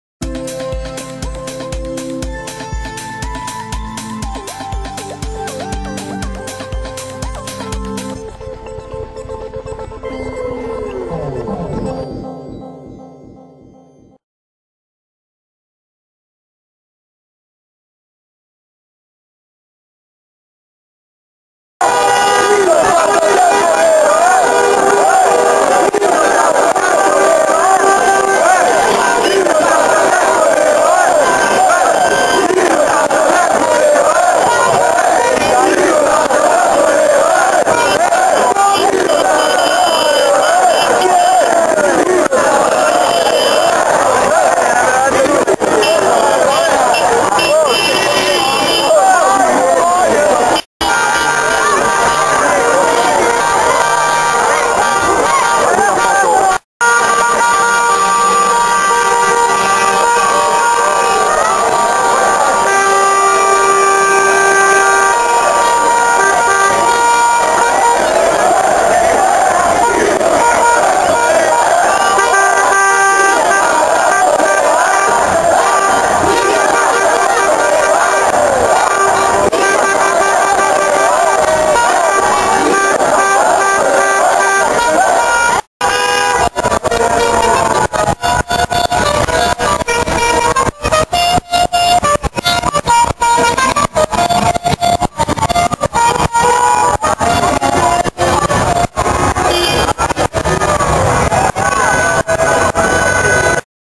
L'Inter vince il campionato, i tifosi scendono in Piazza/2
Tanti i tifosi dell'Inter che sono giunti  in Piazza Prinetti a Merate per festeggiare la vittoria dello scudetto